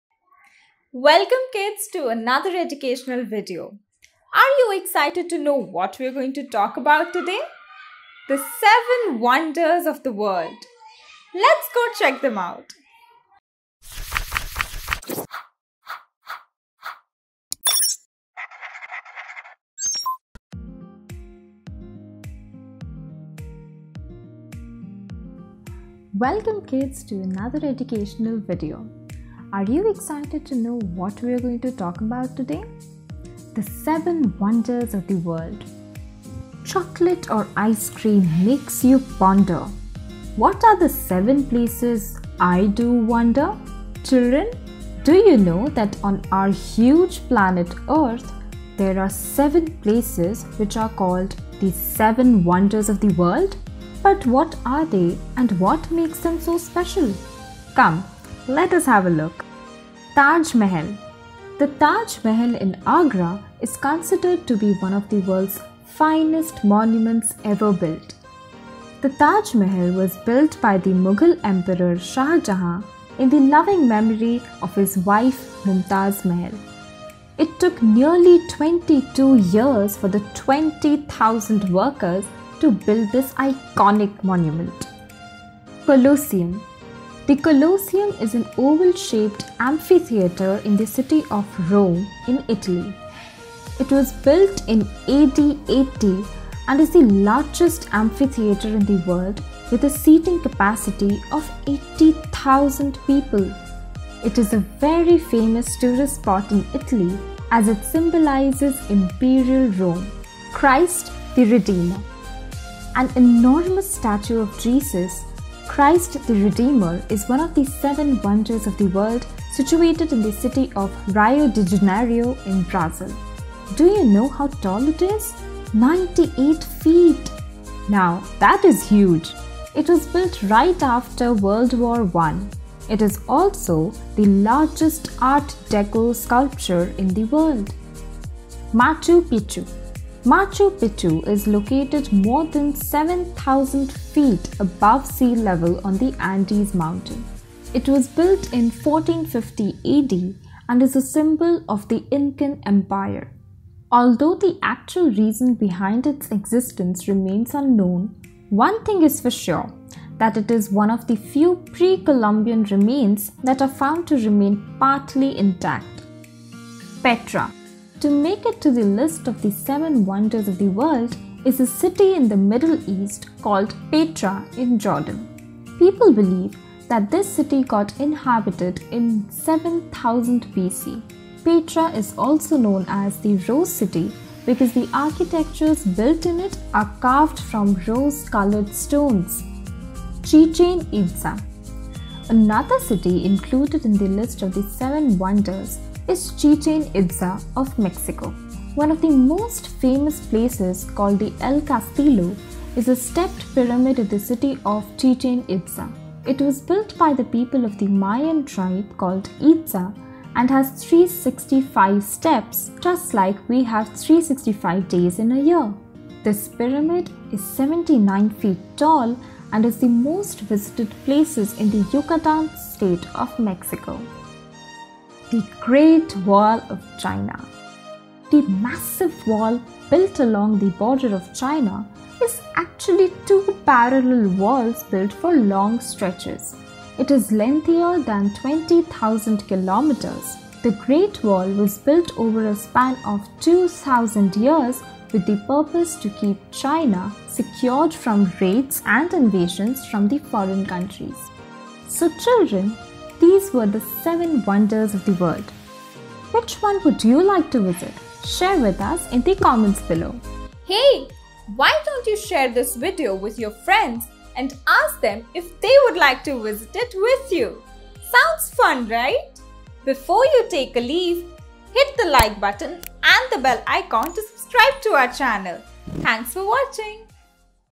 How Many Wonders in the World | Seven Wonders of the World | Educational Videos | Always On Learning
The world is filled with wonders that have survived the test of time and continue to tell a tale filled with grandeur and history. What makes them special? What are the Seven Wonders of the World? Watch this video to know more.